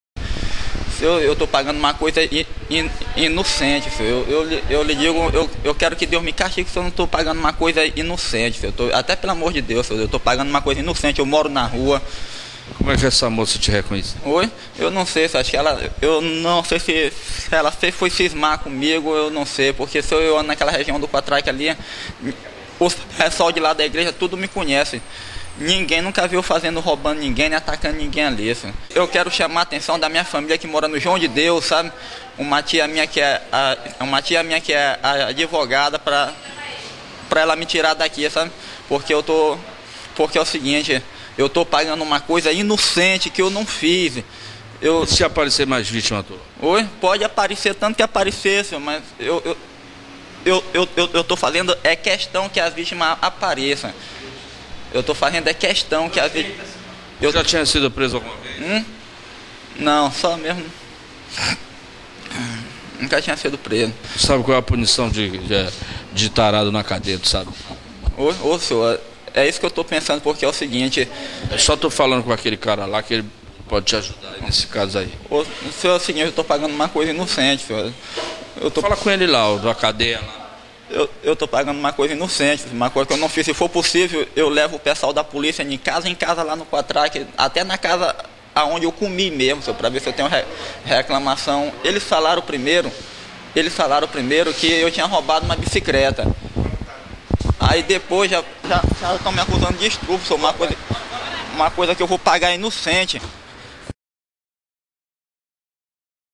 ESTUPRADOR COHATRAC SLZ
A POLÍCIA PRENDEU O ANDARILHO WEBERT GOMES FRANCO.// ELE É APONTADO COMO HOMEM QUE ATACOU VÁRIAS MULHERES NAS REGIÕES DOS COHATRAC´S E JARDIM DAS MARGARIDAS E VIOLENTOU SUAS VÍTIMAS.//A ÚLTIMA DELAS, UMA EVANGÉLICA QUE FOI ATACADA QUANDO ESTAVA INDO PARA A IGREJA, ÀS 6 HORAS DA MANHÃ, HORÁRIO EM QUE ELE COSTUMAVA AGIR.//